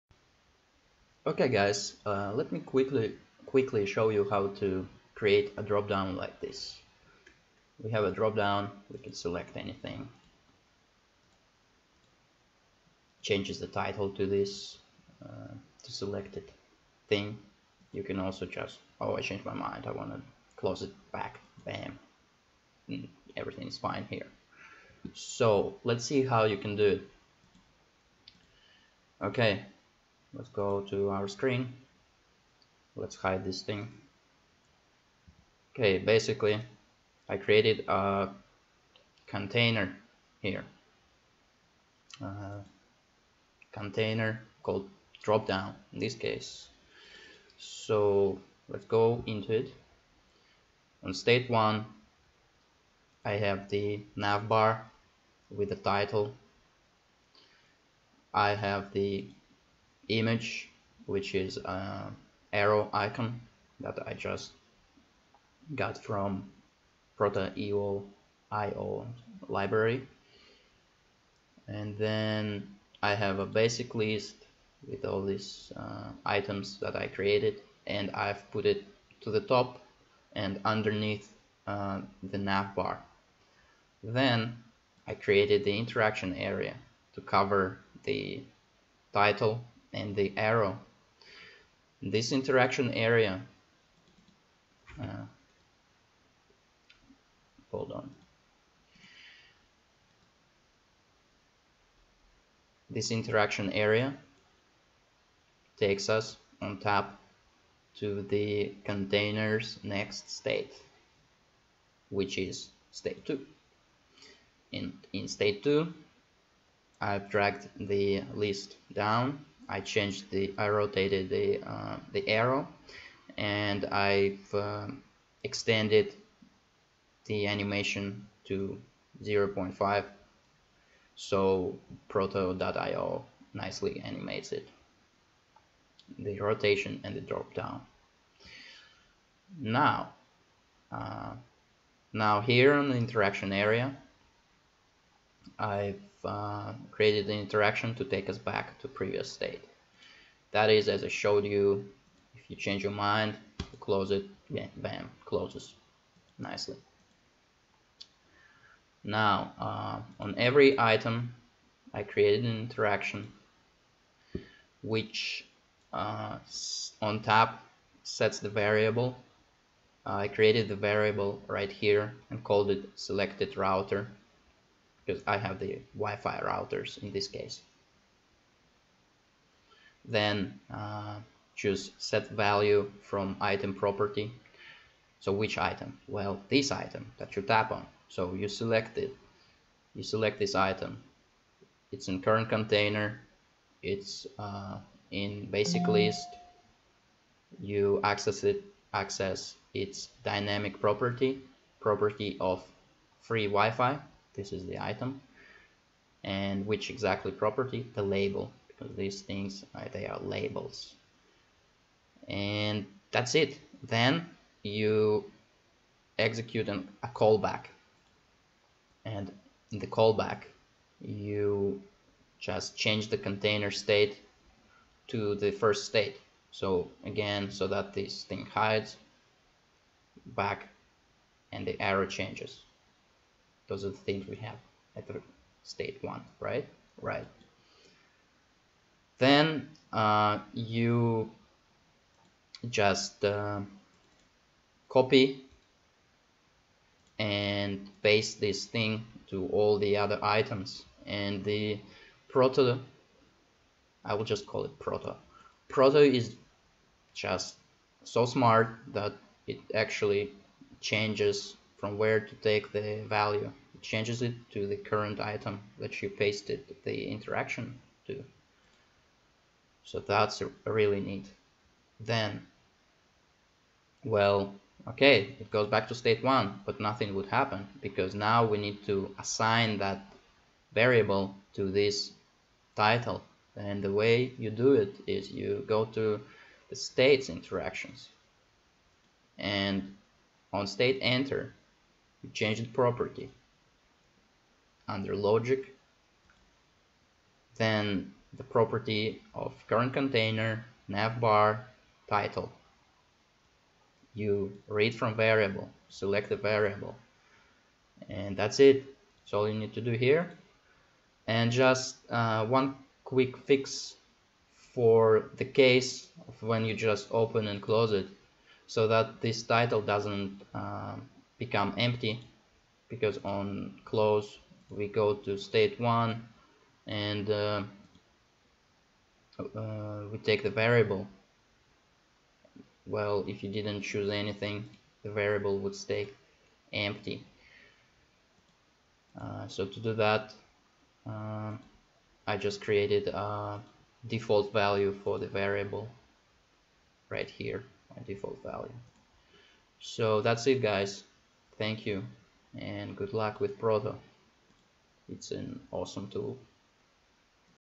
How to create a nav bar dropdown in Proto.io
Here is just a quick tip / tutorial on how to create an interactive dropdown of the mobile navigation bar, using Proto.io

If this helped you, I'll be happy to feel your gratitude in any amount